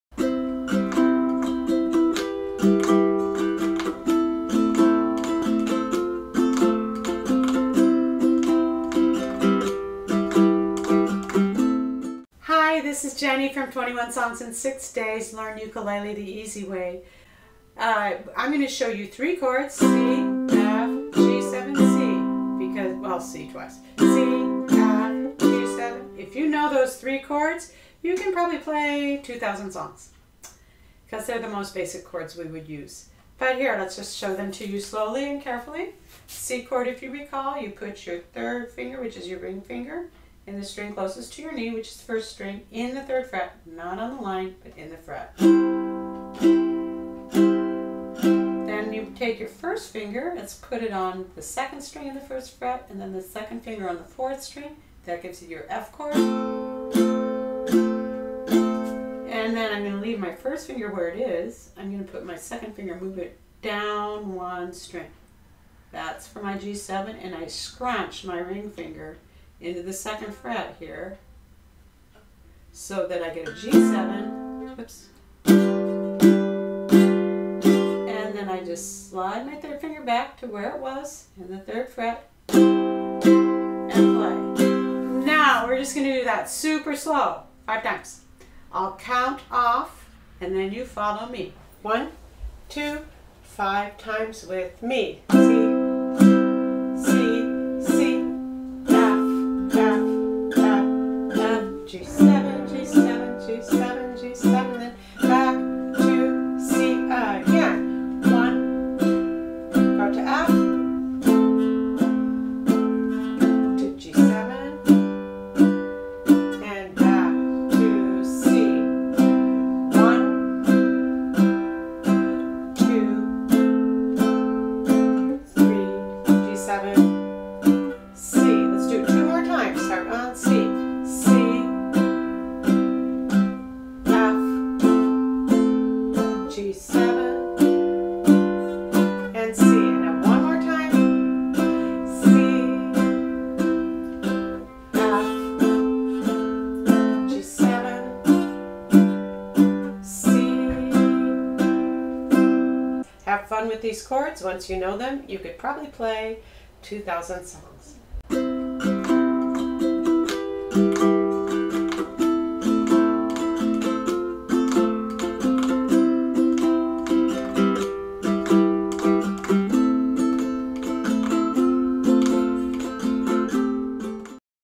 How to Play Ukulele Chords - Etude for C F & G7 - 21 Songs in 6 Days: Learn Ukulele
If you want to learn ukulele, you can check out our book on Amazon titled "21 Songs in 6 Days: Learn Ukulele the Easy Way"